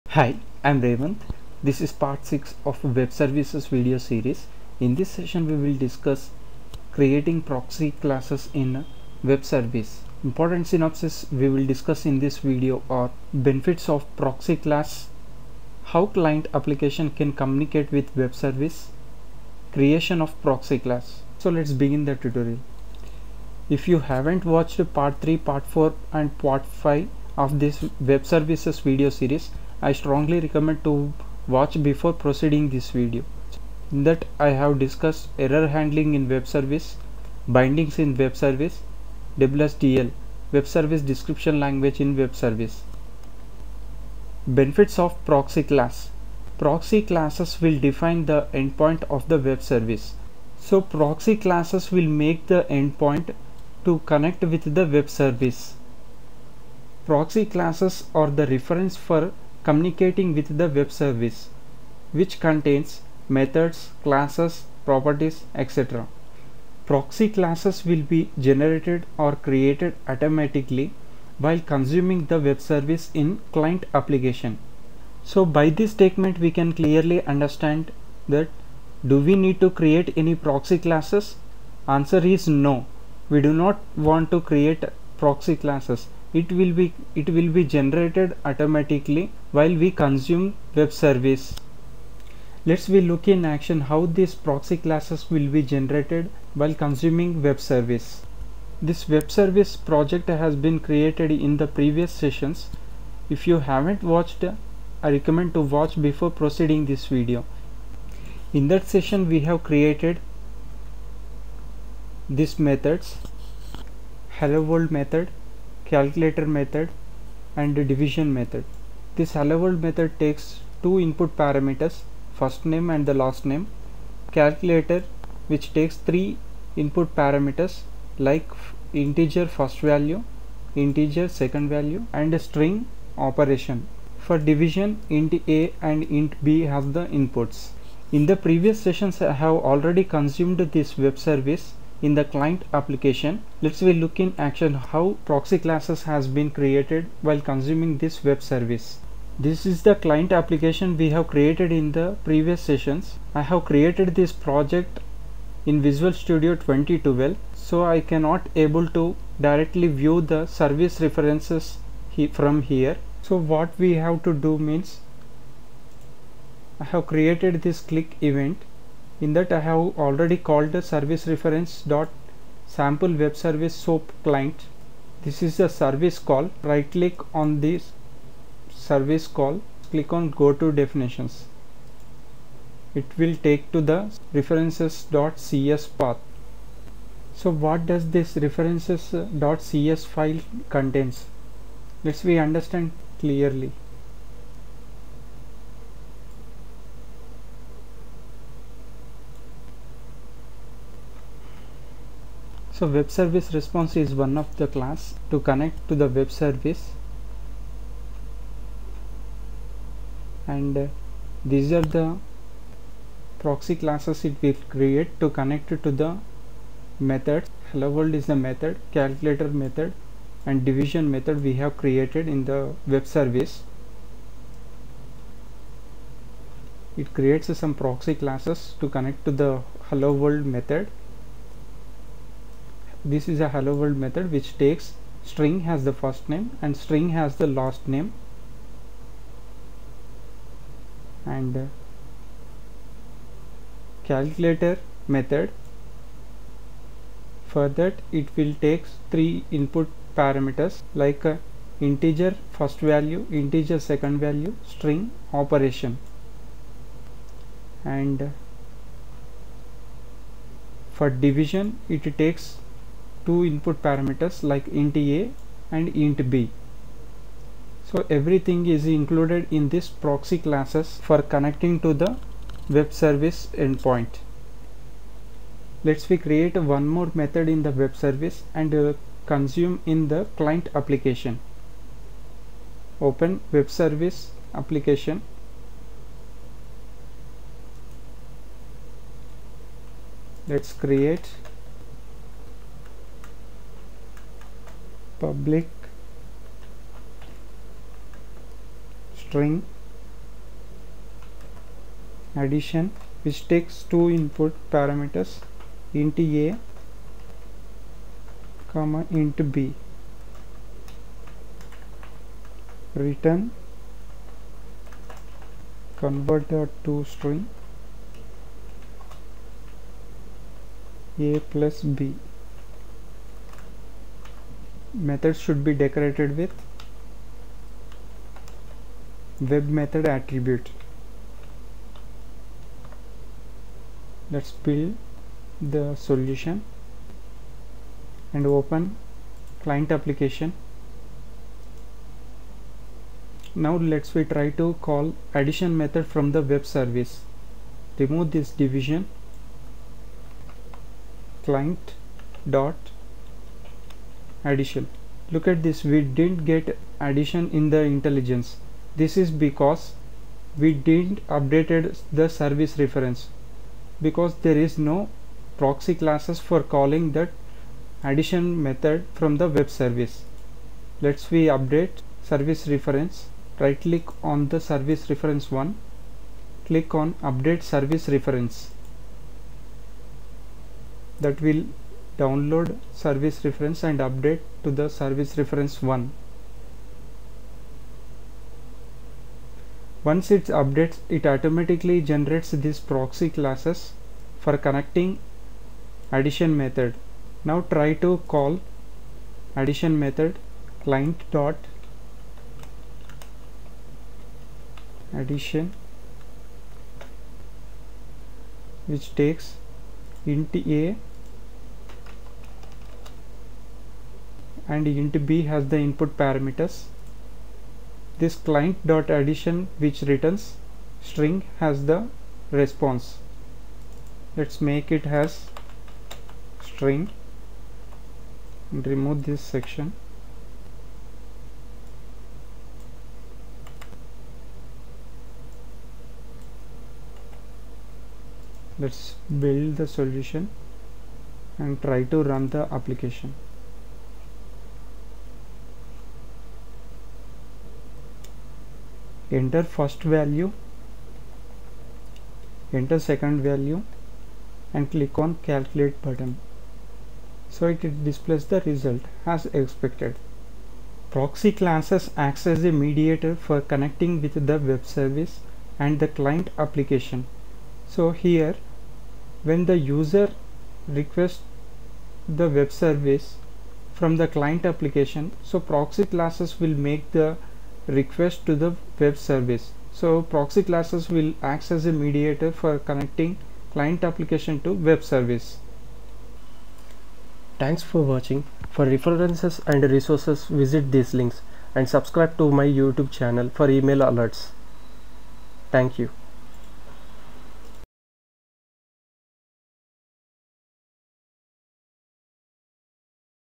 Creating Client Proxy in Web Service Asp.Net C# || Part-6
Learn how to Create Client Proxy Classes in Web Service Asp.Net C# || Part-6

Description: ( Open to view the description )
* Proxy Classes will define the endpoint of the Web Service.
* Proxy Classes are the reference for communicating with the Web Service. Which contains methods, classes, properties etc.
* Proxy Classes will be generated or created automatically while consuming the Web Service in Client application.
* Proxy Classes acts as a mediator for connecting with the Web Service and Client Application.

Important synopsis discussed in this video
1. Benefits of Proxy Class
2. How client application can communicate with Web Service
3. Creation of Proxy